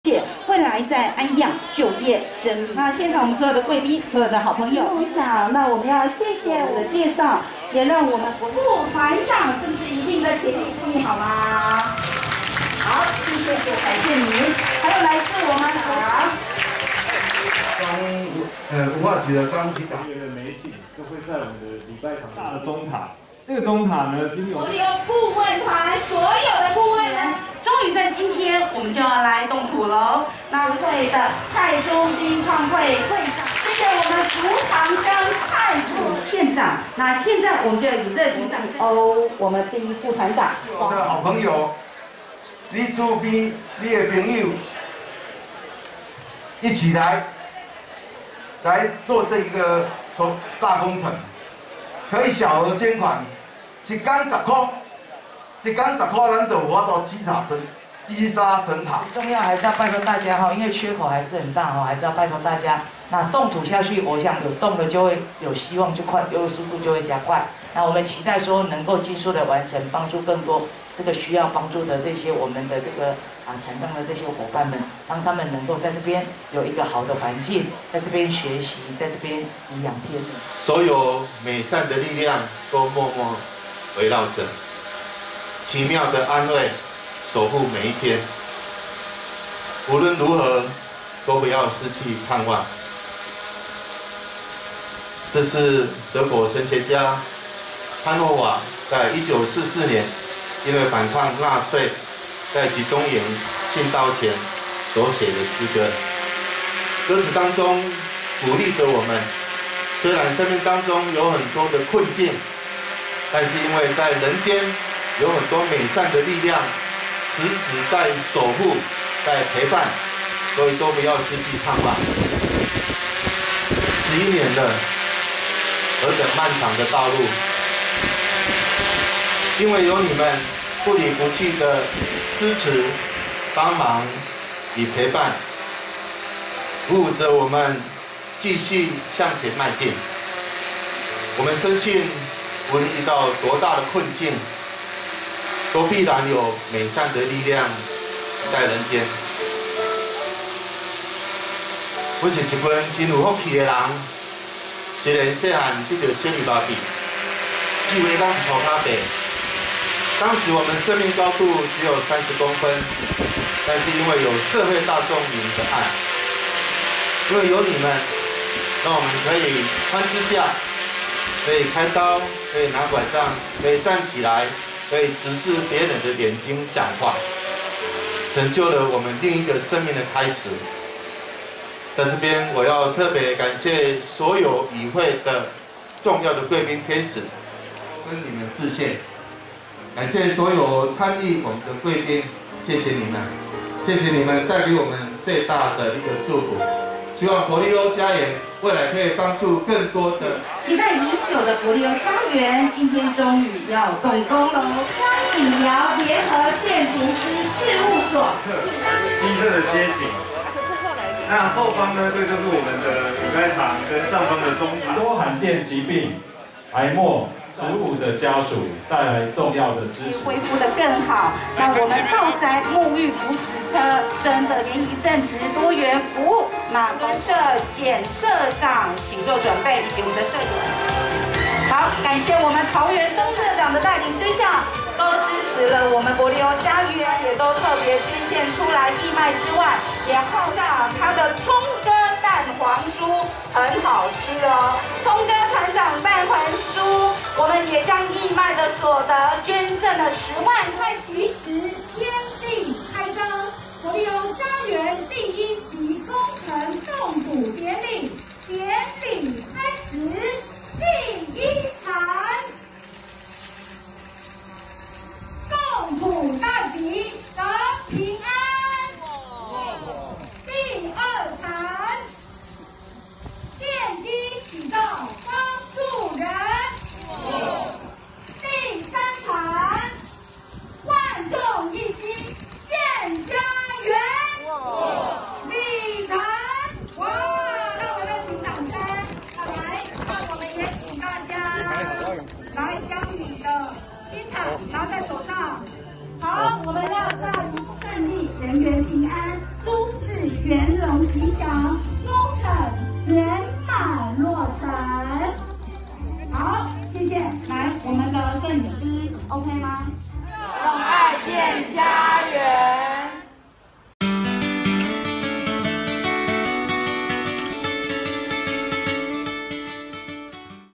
伯立歐家園百人揮鏟動土 萬眾一心齊心建家園
伯立歐家園百人揮鏟動土　萬眾一心齊心建家園 【記者林明佑／竹塘鄉報導】為打造一處讓身心障礙者年老時安身立命的所在，彰化縣喜樂小兒麻痺關懷協會於11年前開始籌建伯立歐家園計畫，今(24)日在眾人的祝福下，於竹塘鄉東陽路二段的伯立歐家園預定地，舉行「萬眾一心‧齊心建家園-伯立歐家園動土感恩禮拜」，接近500人一起揮動金鏟，共同見證這歷史的一刻，場面壯觀、溫馨感人。@changhuanews